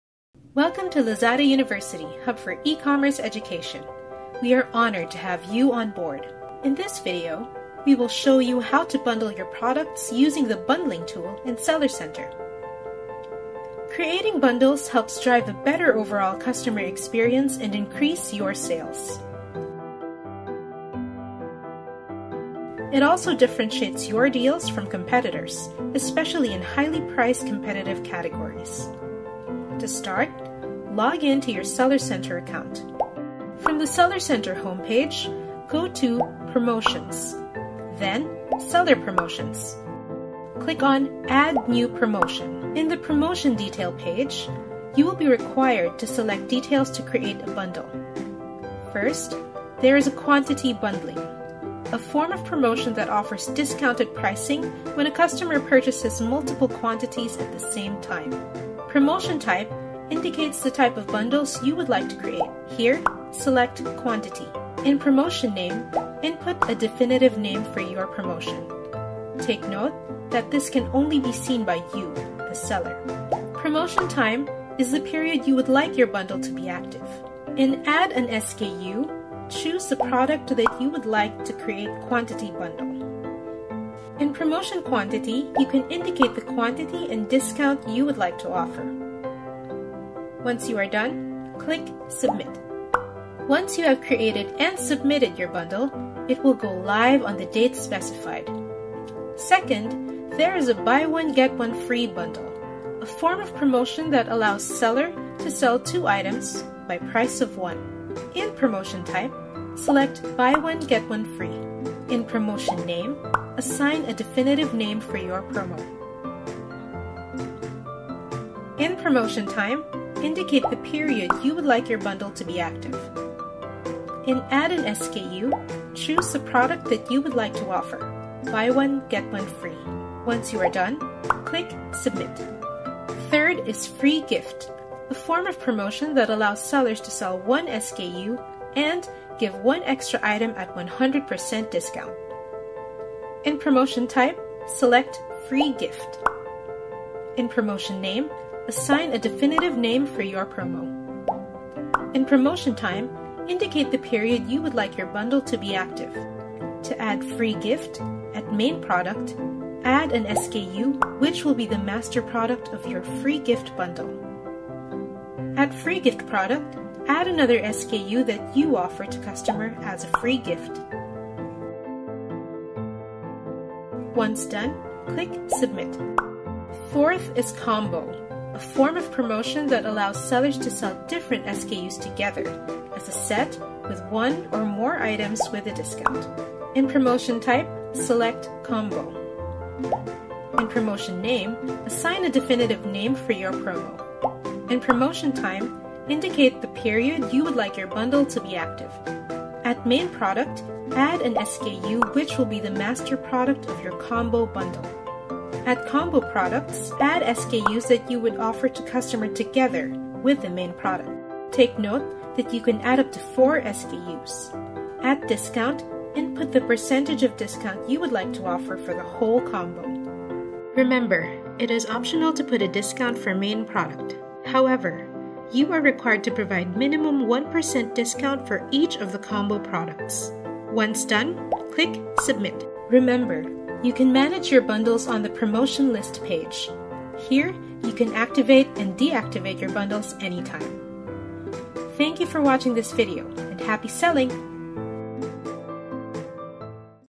How to create bundles in Seller Center
Learn how to bundle your products using bundling tool